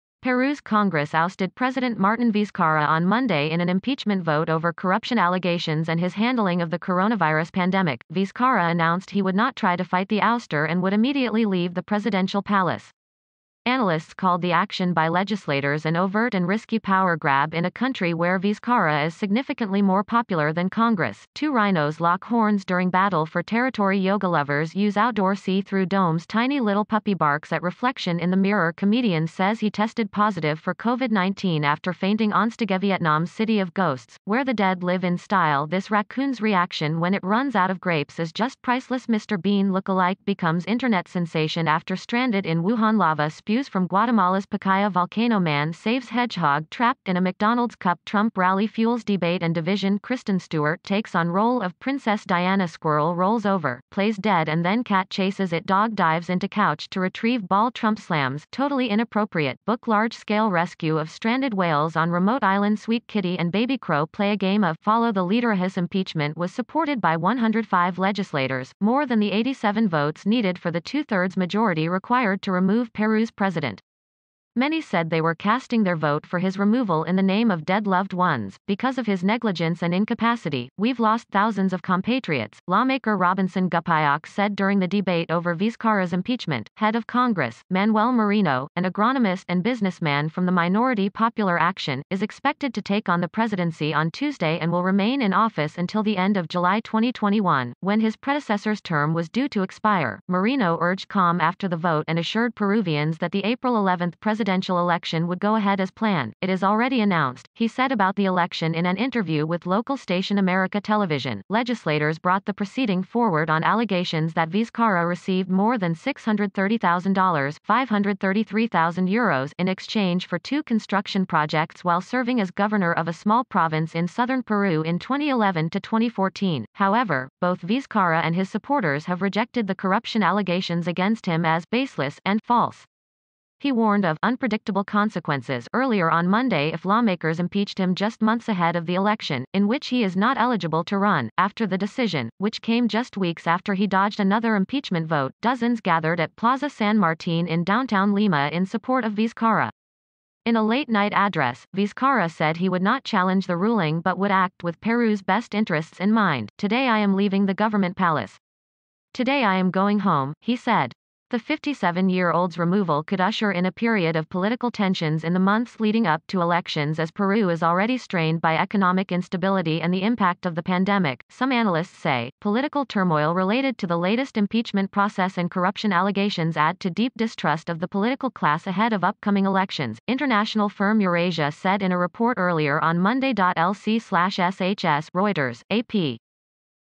Peruvian President Martin Vizcarra ousted by Congress over corruption charges
Peru's Congress ousted President Martin Vizcarra on Monday in an impeachment vote over corruption allegations and his handling of the coronavirus pandemic.Vizcarra announced he would not try to fight the ouster and would immediately leave the presidential palace. Analysts called the action by [...]